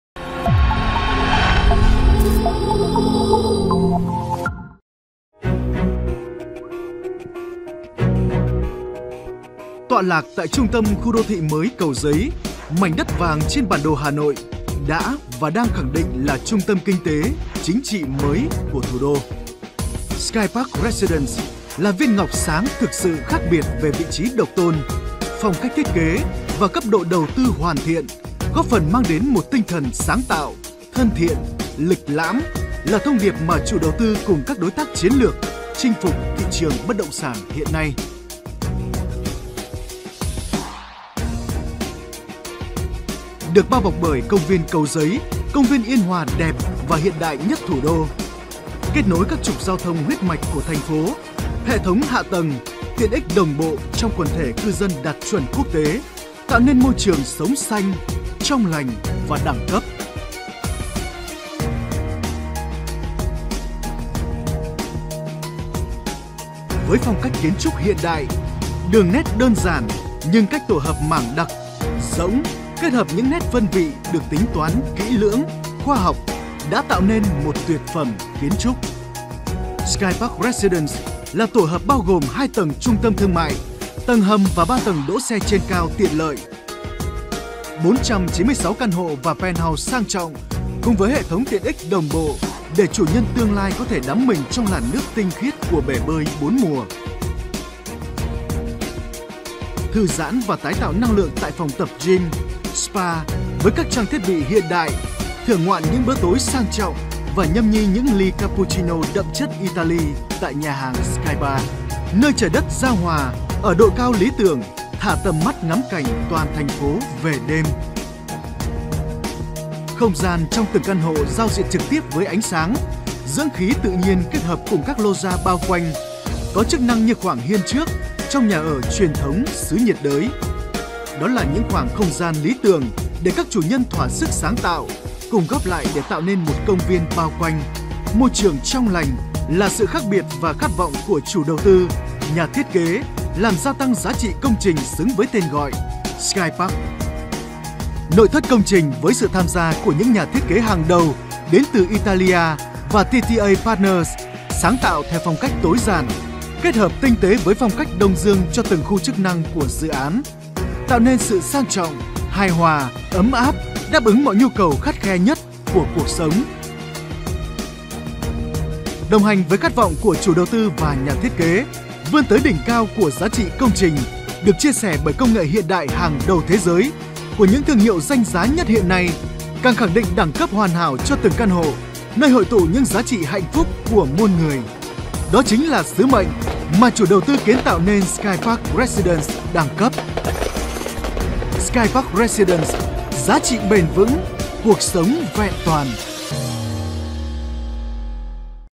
#1 Bảng giá Chung cư Sky Park Residence Tôn Thất Thuyết - Cầu Giấy
Video tổng quan và Chung cư Sky Park Residence Tôn Thất Thuyết trung tâm quận Cầu Giấy.